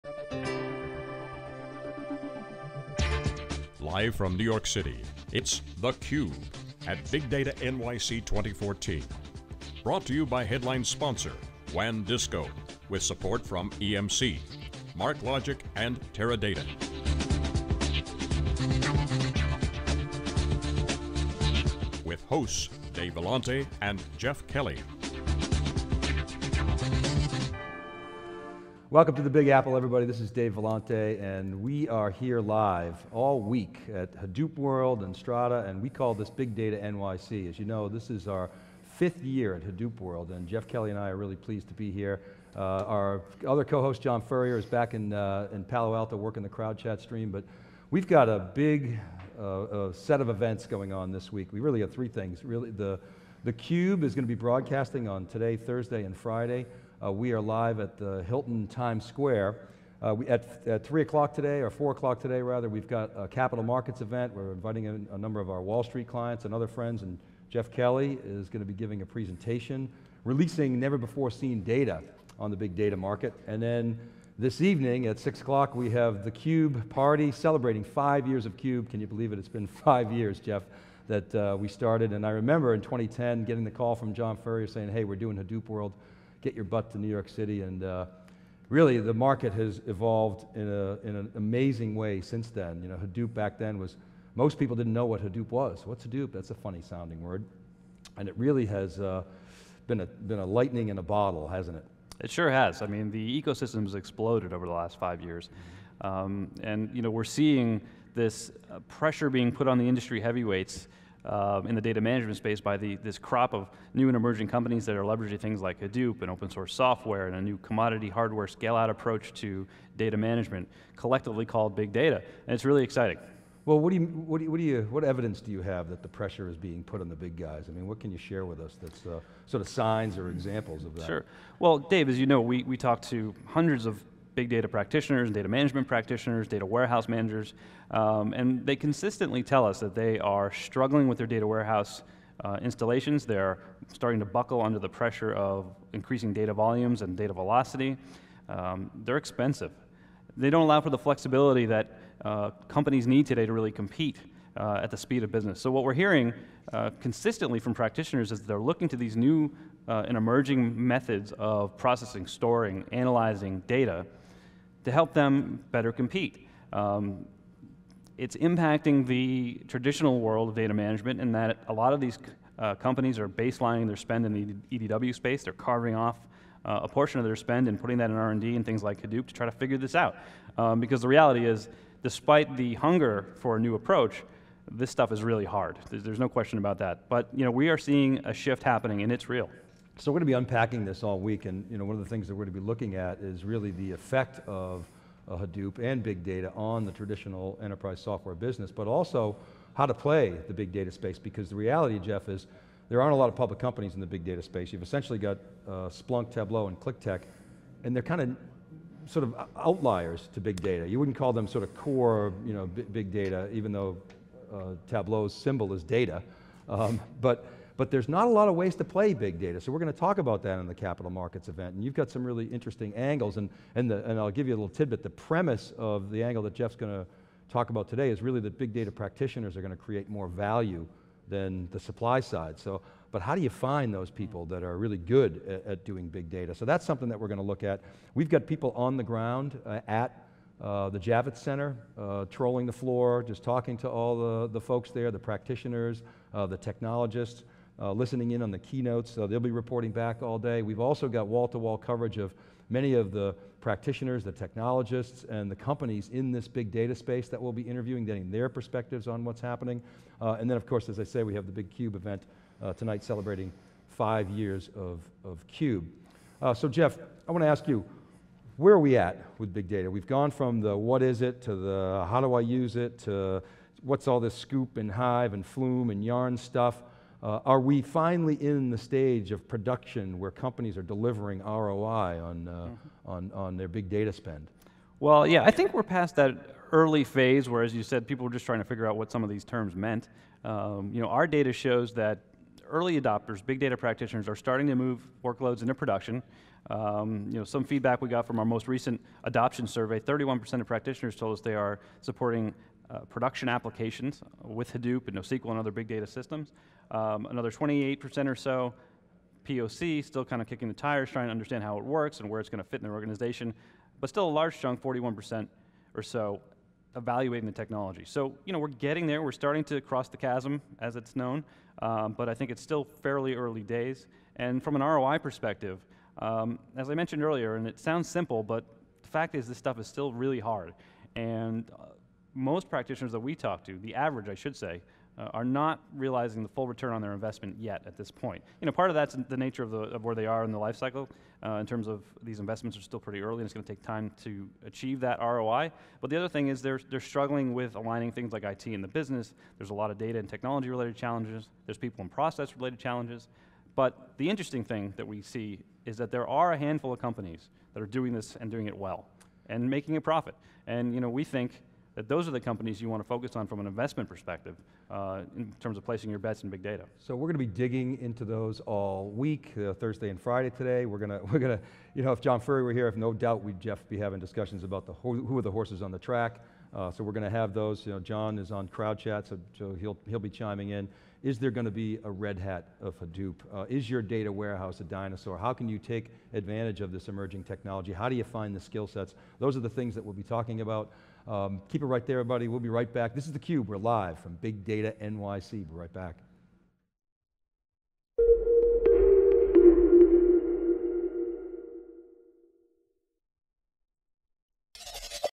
Intro - BigDataNYC 2014 - theCUBE
Intro, BigDataNYC 2014 with Dave Vellante and Jeff Kelly

Wrapping up their coverage of the Big Data NYC conference, Jeff Kelly and Dave Vellante discussed some noteworthy patterns emerging in the Big Data enterprise landscape. As storage costs increase and budgets remain flat, enterprises are baselining data warehouse spending in favor of Hadoop experimentation. In fact, theCUBE hosts explained how practitioners are only spending 30 percent of what they used to on their data warehouse, according to Wikibon.org research. Though the enterprise data warehouse (EDW) didn’t live up to its potential, said Kelly, the jury is still out on whether Big Data and Hadoop will be able to deliver on promises.

Kelly and Vellante drew on Wikibon survey results from Kelly’s conference presentation to show that the enterprise is increasingly interested in Hadoop and Big Data:

    1. 25 percent of surveyed Hadoop practitioners pay vendors for a subscription to their commercial distribution, which includes support. TheCUBE hosts expect this number to increase as more practitioners begin production deployments.

    2. More than half of practitioners are past the evaluation phase in their big data deployments: 28 percent of survey respondents are conducting a pilot or proof of content and 31 percent are in production deployment.

    3. 38 percent of respondents use NoSQL databases and 36 precent use Hadoop. These numbers are important, explained Kelly, because those to technologies are “foundational” in the “digital fabric.” Along with Cloud and Infrastructure as a Service, Hadoop is “becoming the new operating service in a data-centric world.”

    4. 92 percent of respondents believe that Big Data analytics is more than “a buzzword.” Kelly suggested that this statistic has to do with Big Data analytics success stories, like Facebook and Google, which inspire other companies to take the phenomenon seriously.

    5. 65 percent of Big Data practitioners who responded to the Wikibon survey said they had already shifted resources from EDW to Big Data and Hadoop. Another 35 percent said they planned to do so before the end of the year.

These stats, said Vellante, suggest the start-up world of Hadoop is quickly combining with the corporate enterprise culture. TheCUBE hosts predict companies will “operationalize” Hadoop by figuring out how to